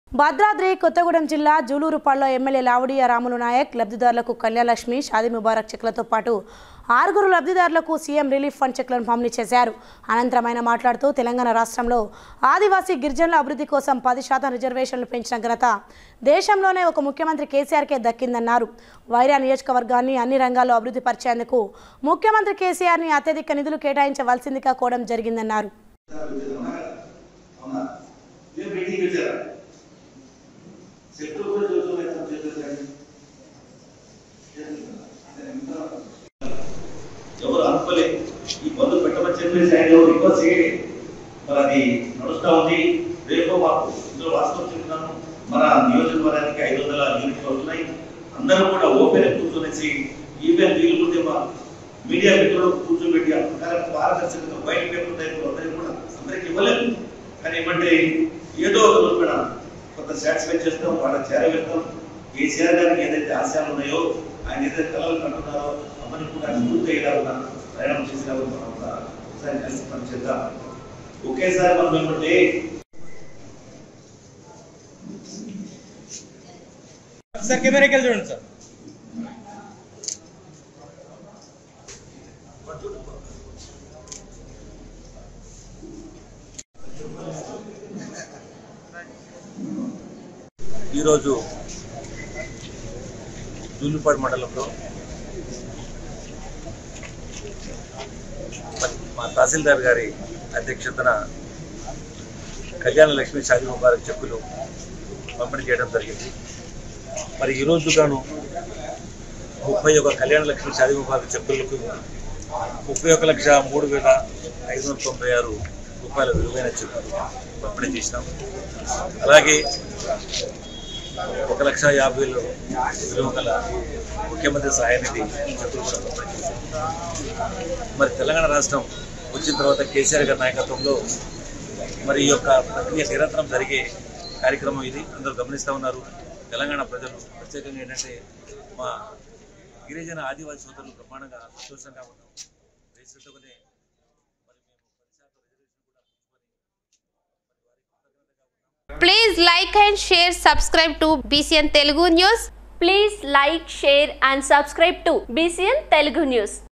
లబ్ధిదారులకు సీఎం రిలీఫ్ ఫండ్ చెక్కుల పంపిణీ : MLA లావుడియా రాముల నాయక్
భద్రాద్రి కొత్తగూడెం జిల్లా :జూలూరుపాడు లో MLA లావుడియా రాముల నాయక్  ,లబ్ధిదారులకు కళ్యాణలక్ష్మి షాదీముబారక్  చెక్కులతోపాటు, ఆరుగురు లబ్ధిదారులకు సీఎం రిలీఫ్ ఫండ్ చెక్కులను  పంపిణీ చేశారు.
అనంతరం ఆయన మాట్లాడుతూ తెలంగాణ రాష్ట్రంలో ఆదివాసి గిరిజనుల అభివృద్ధి కోసం 10 శాతం రిజర్వేషన్లు పెంచిన ఘనత దేశంలోనే ఒక ముఖ్యమంత్రి కేసీఆర్ కే దక్కిందన్నారు.
వైరా నియోజకవర్గాన్ని అన్ని రంగాల్లో అభివృద్ధిపరిచేందుకు ముఖ్యమంత్రి కేసీఆర్ ని అత్యధిక నిధులు కేటాయించవలసిందిగా కోరడం జరిగిందన్నారు.
Bhadradri Kothagudem District: In Julurupadu, MLA Lavudiya Ramula Naik distributed Kalyanalakshmi Shadimubarak checks to beneficiaries and CM Relief Fund checks to six beneficiaries.
Later, he said that the credit of increasing 10 percent reservation for the development of adivasi tribes in Telangana state has been given to KCR, a chief minister in the country.
He said that Chief Minister KCR has been asked to allocate maximum funds for the development of Vaira constituency in all fields.